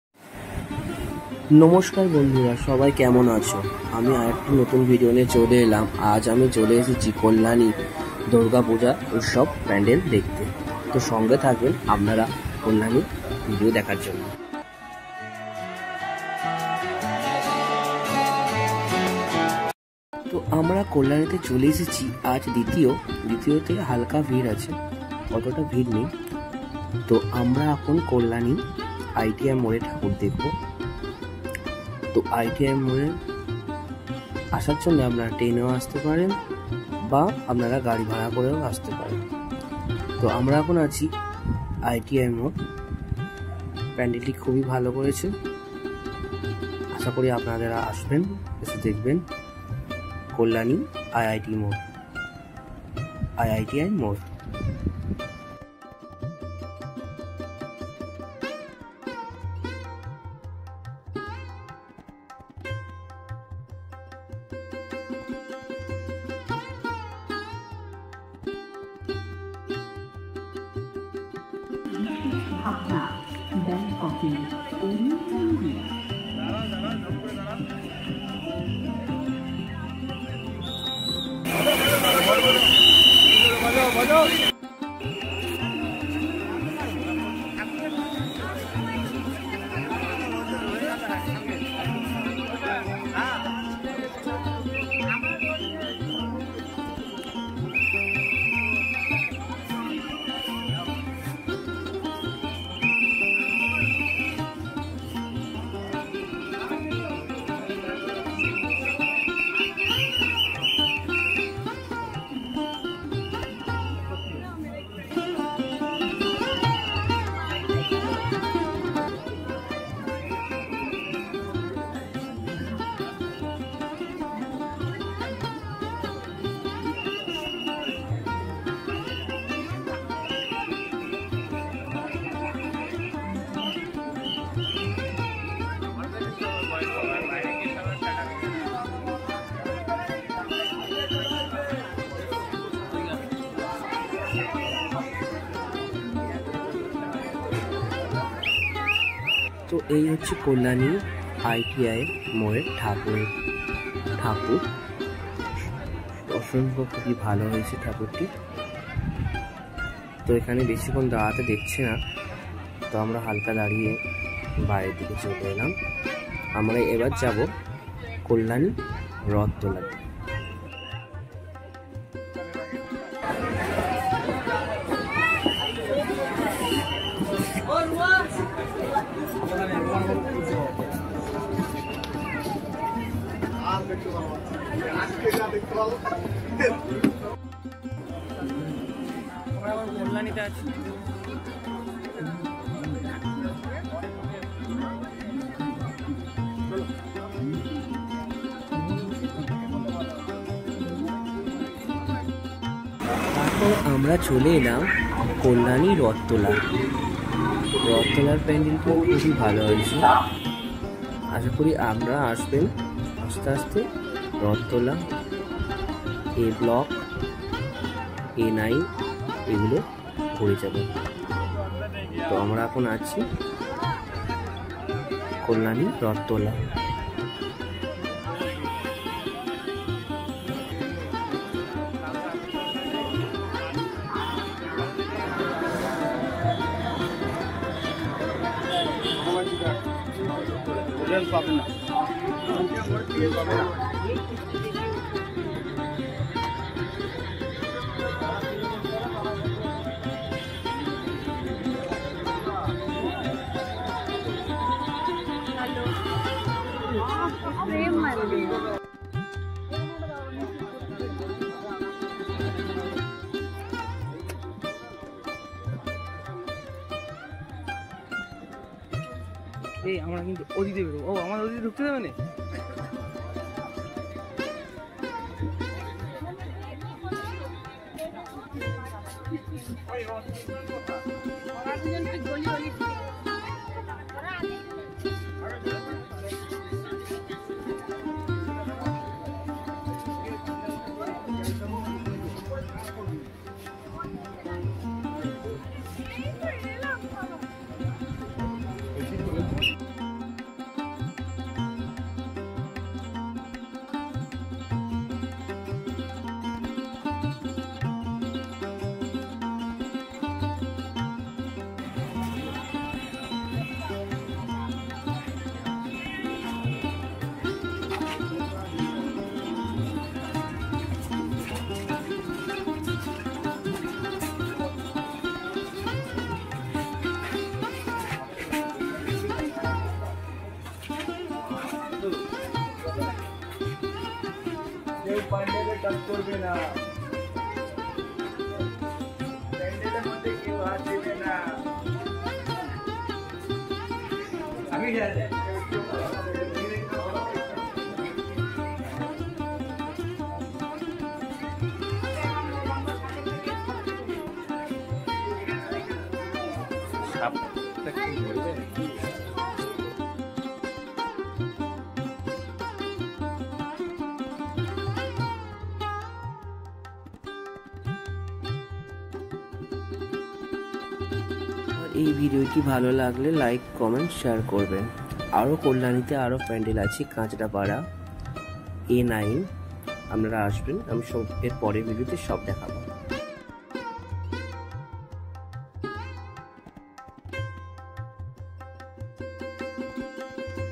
কল্যানীর সেরা 3 দুর্গাপূজা কোথায়? কিভাবে দেখবেন Kalyani Durga Puja Vlog 2024| ITI More A9 Square
Kalyani ITI Mor Durga Puja Pandal || কল্যাণী ITI Durga Puja || Kalyani Durga Puja Pandal

Digital Time Suman

tour & travel

আপনাদের পাশে সব সময় পাবেন ডিজিটাল টাইম সুমনকে 💌📢